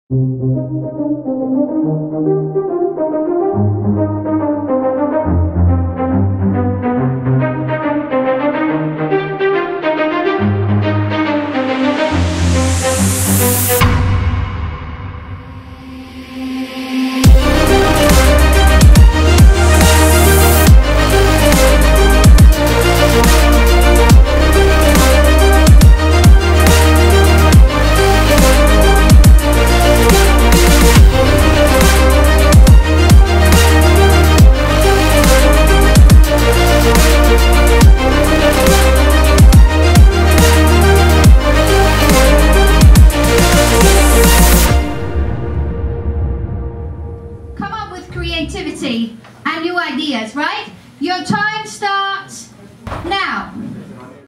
Queen Marry university - Idea Race Event
An event featuring a competition of innovative ideas with judges and professors from Queen Mary University of London. The event included presentations by student groups showcasing their creative ideas, and ultimately, one group was awarded a prize and received university support to implement their idea

Video by : LBB Channel
Taketones.com